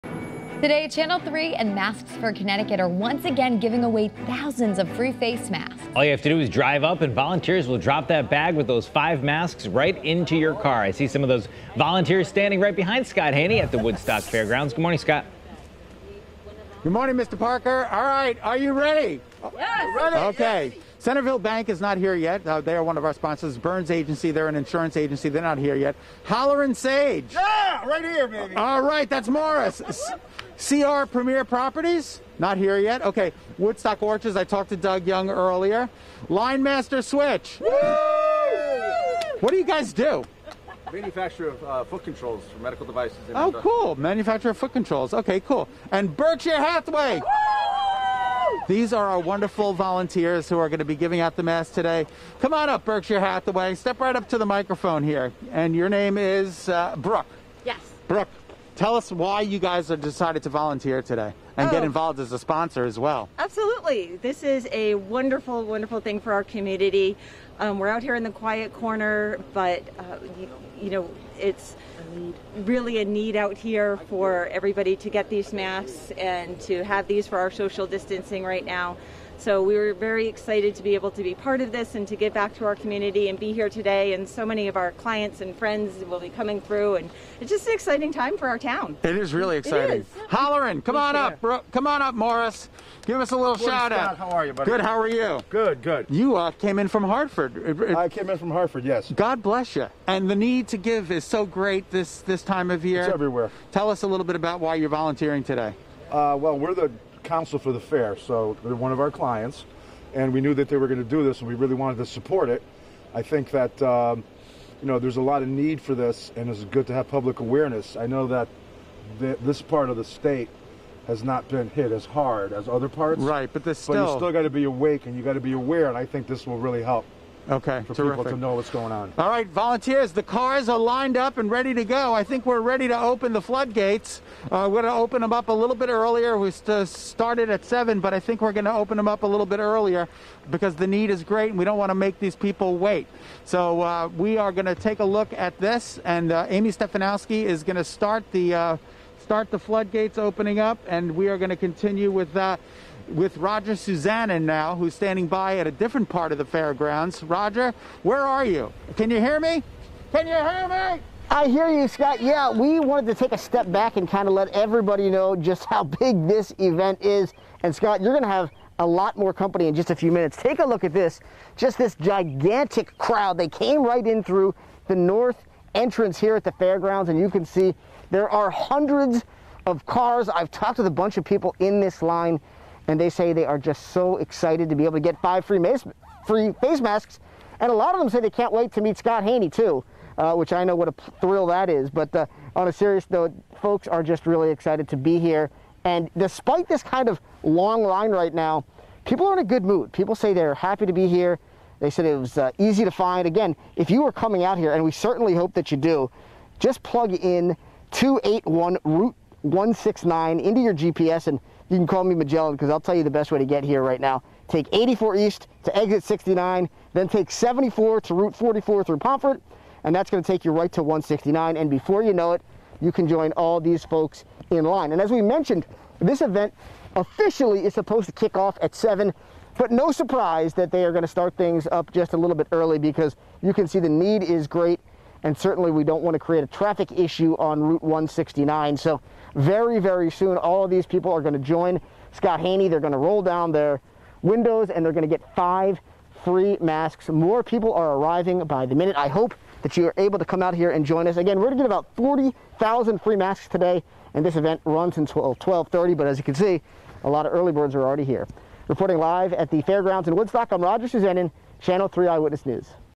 VIDEO: Vehicles line up for 9th Masks for CT event
Drivers lined up early for Channel 3's 9th Masks for CT event at the Woodstock Fairgrounds.